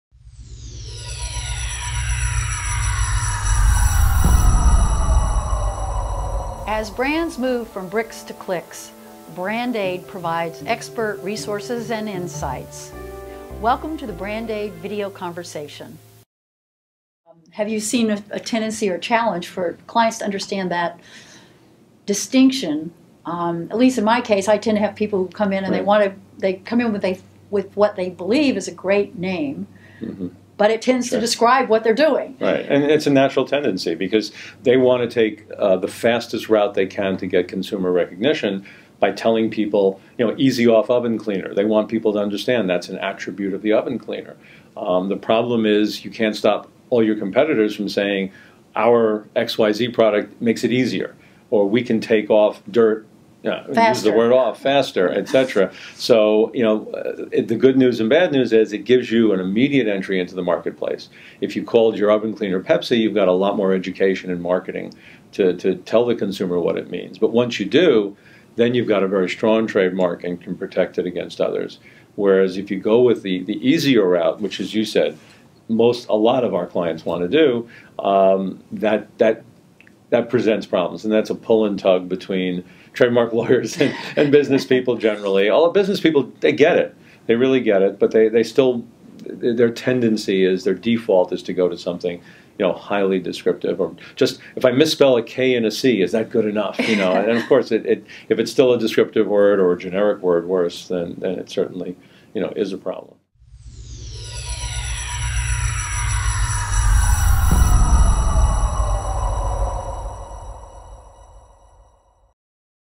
What Is A Descriptive Trademark?
Trademark lawyers Bill Finkelstein and Cheryl Hodgson discuss the problem with using and trying to register a descriptive term as a trademark